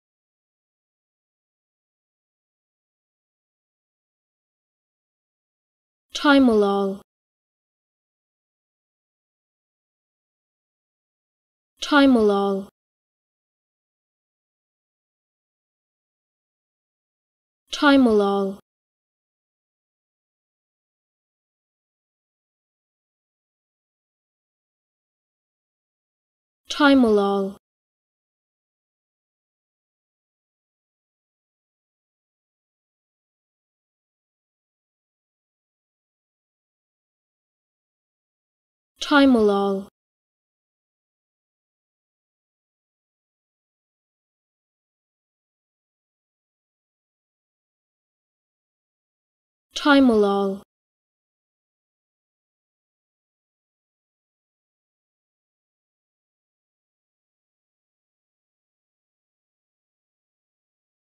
How To Say Timolol
Learn how to say Timolol with EmmaSaying free pronunciation tutorials.